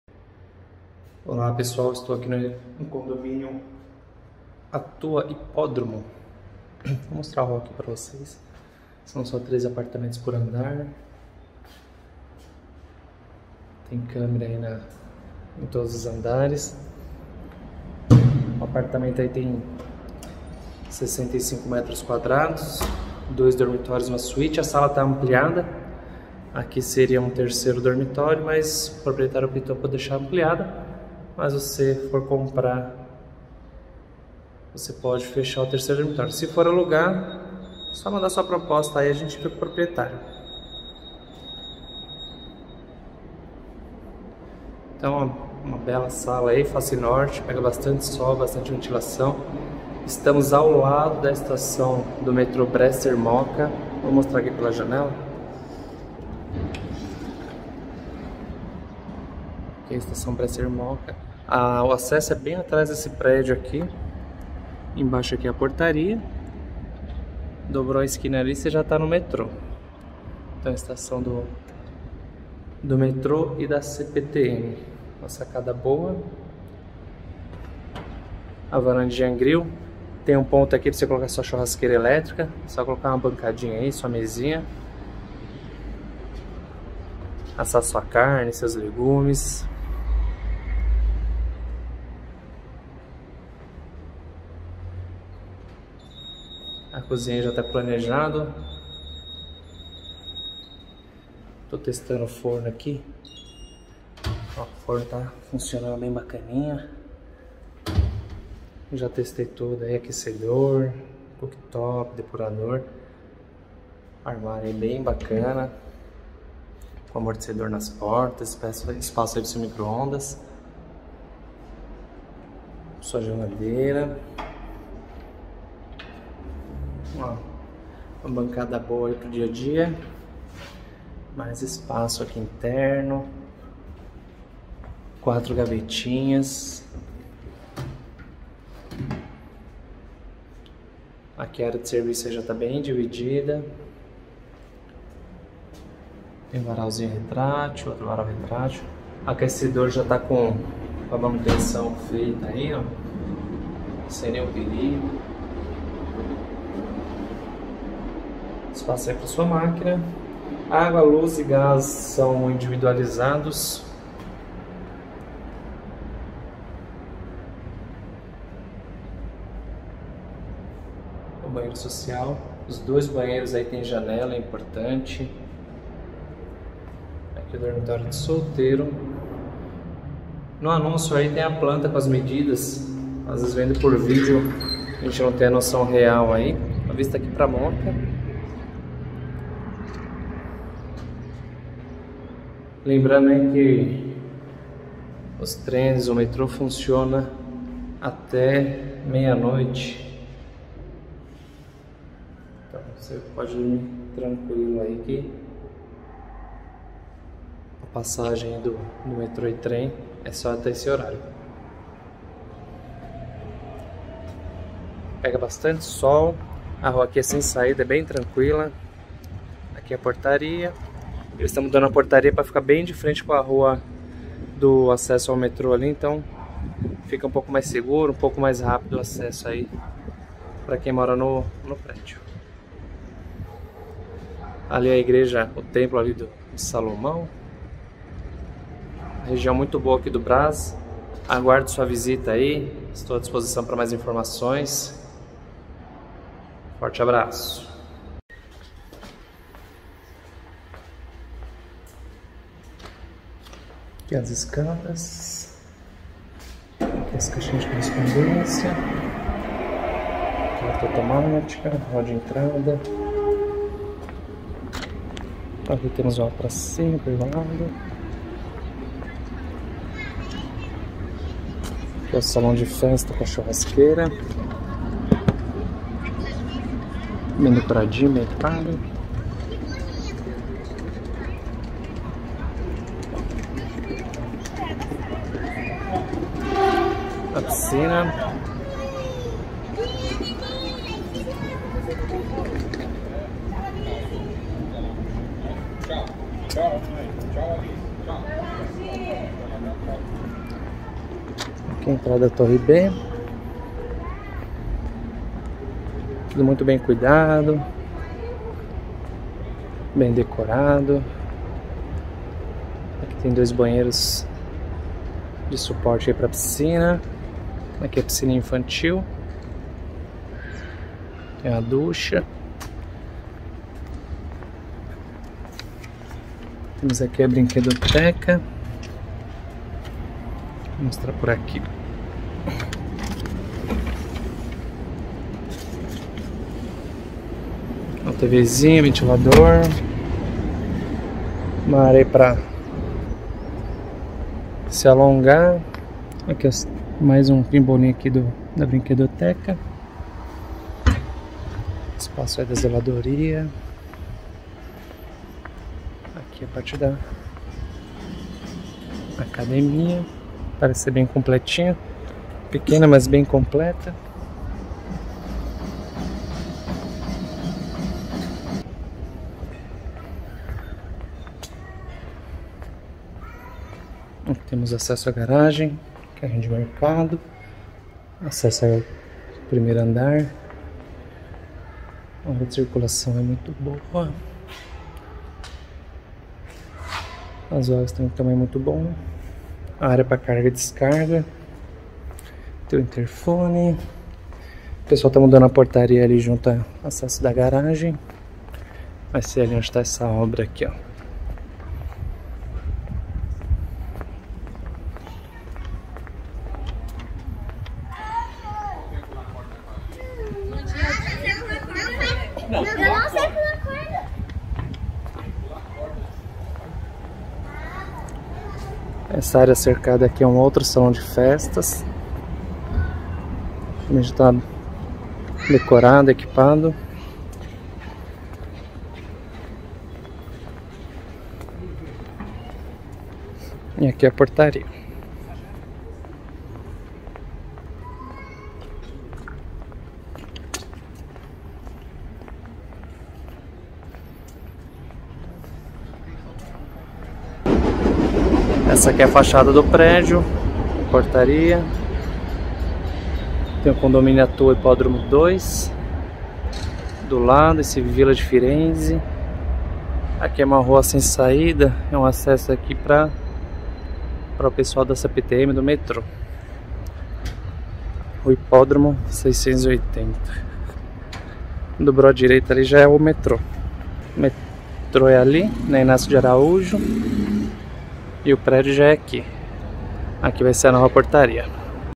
APARTAMENTO PARA VENDA E LOCAÇÃO PRÓXIMO METRÔ BRÁS, SÃO PAULO
CONDOMÍNIO ATUA HIPÓDROMO
Excelente apartamento para alugar/vender ao lado do Metrô Brás.

Imóvel possui 65m² bem distribuídos em:
- 2 Dorms. (1 suíte) (reversível 3º dormitório)
- Sala 2 ambientes ou 3 ambientes
- Varanda
- Cozinha com armário planejado, cooktop, forno e depurador de ar
- 1 vaga de garagem.

Aluguel R$ 2.250,00
Condomínio R$ 525
IPTU R$ 17,51
Seguro Incêndio R$ 30 +/-
Pacote de locação: R$ 2.822,51/mês

Condomínio Atua Hipódromo, lazer completo!! Churrasqueira, piscina, playground, salão de jogos, salão de festas, quadra poliesportiva e muito mais!!

Localização privilegiada, fica ao lado do Metrô Bresser-Mooca, à 5 min. do Supermercado EXTRA-MOOCA e da Universidade São Judas, fácil acesso as Avenidas Celso Garcia, Radial Leste e Marginal Tietê. Próximo estação Brás da CPTM. Faça suas compras no Brás fica a apenas 5 minutos.